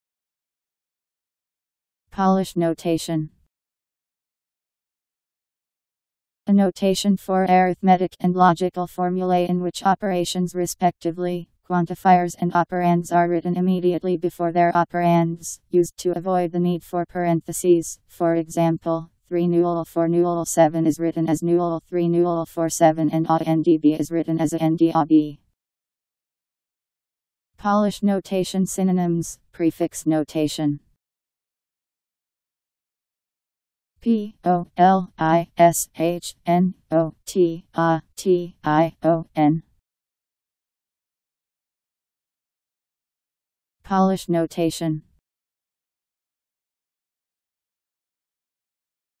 Polish notation Meaning
Video shows what Polish notation means. A notation for arithmetic (and logical) formulae in which operations (respectively, quantifiers and operands) are written immediately before their operands, used to avoid the need for parentheses; for example, 3 * (4 + 7) is written as * 3 + 4 7 and A AND B is written as AND A B.. Polish notation synonyms: prefix notation.  Polish notation Meaning. How to pronounce, definition audio dictionary. How to say Polish notation. Powered by MaryTTS, Wiktionary